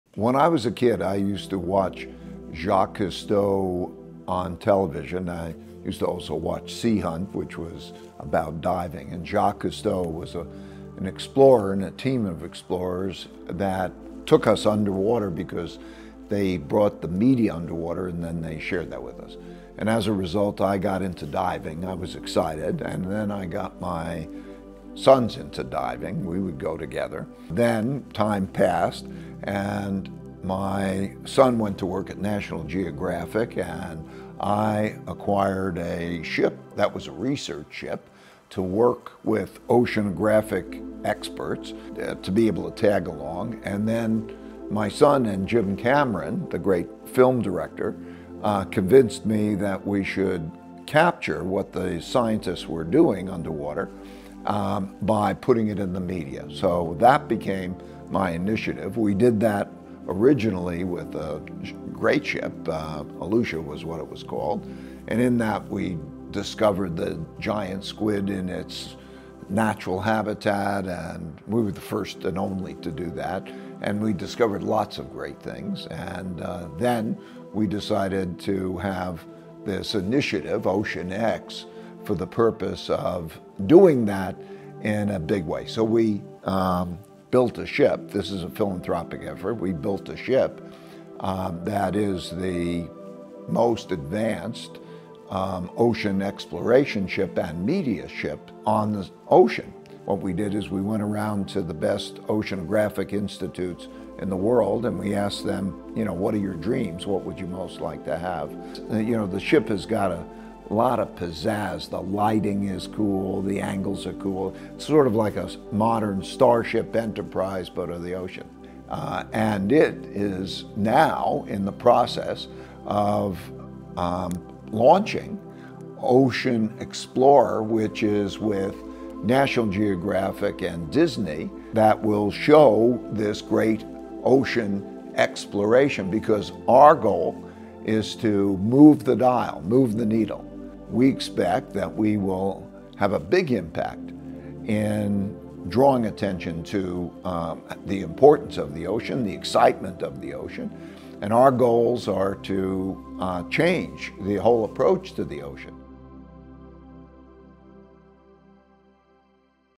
Why I Founded OceanX
I co-founded @OceanX with my son Mark because I’m passionate about the ocean and ocean exploration and want to share it with you.

To bring you along on this journey, I’ve partnered with National Geographic, the BBC, and James Cameron on the OceanXplorers series, which you can watch now on the NG Channel, Disney+, and Hulu.